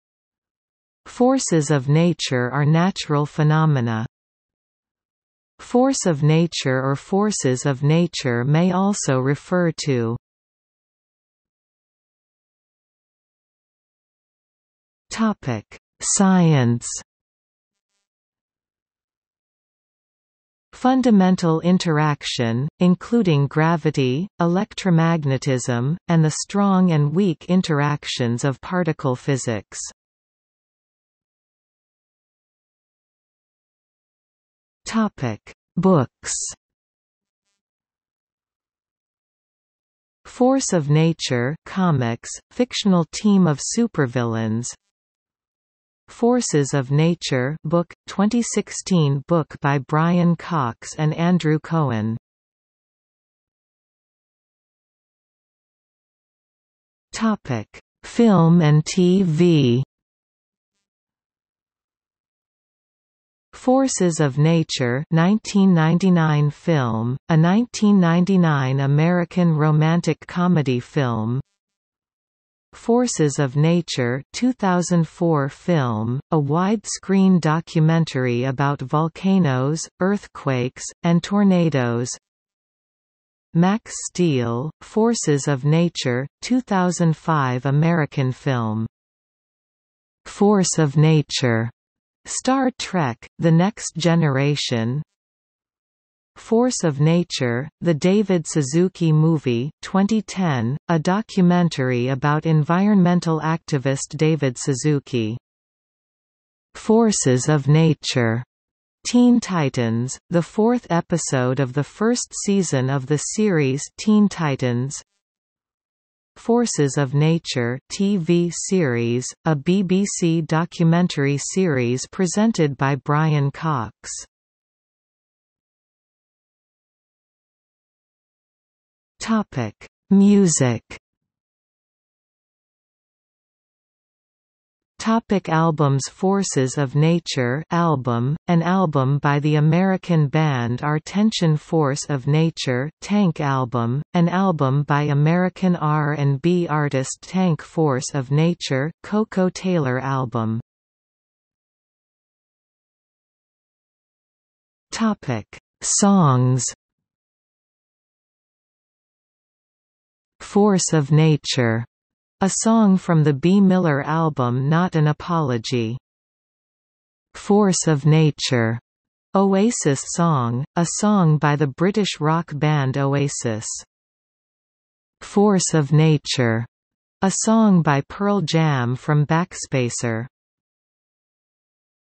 Force of nature (disambiguation) | Wikipedia audio article
00:00:11 1 Science
00:00:31 2 Books
00:00:53 3 Film and TV
00:02:05 4 Music
00:02:14 4.1 Albums
00:02:31 4.2 Songs

- Socrates

SUMMARY
Forces of nature are natural phenomena.
Force of Nature or Forces of Nature may also refer to: